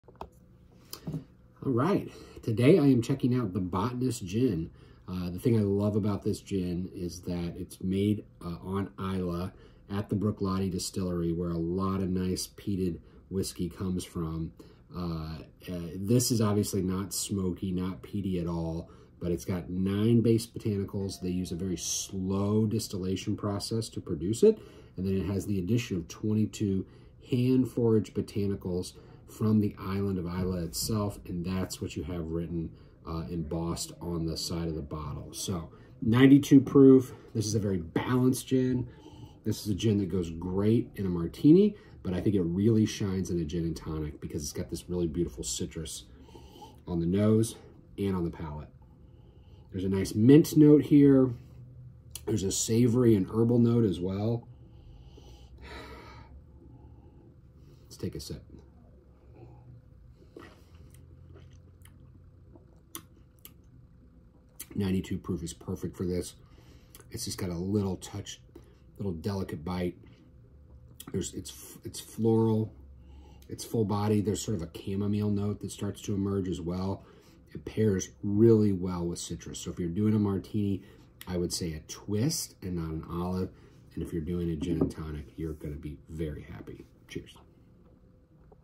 The Botanist Islay Dry Gin
🍸 The Botanist Islay Dry Gin - The Botanist Gin is a dry style gin distilled on Islay at the Bruichladdich distillery using 9 base botanicals and 22 additional hand-foraged island botanicals. The profile is sweet and spicy with mint, savory herbs, and a touch of chamomile. The palate is citrus forward with delicate herbal and floral notes. This is SLOWLY distilled and the result is an elegant, balanced, and flavorful gin that doesn’t stand in the way of a great martini or G&T.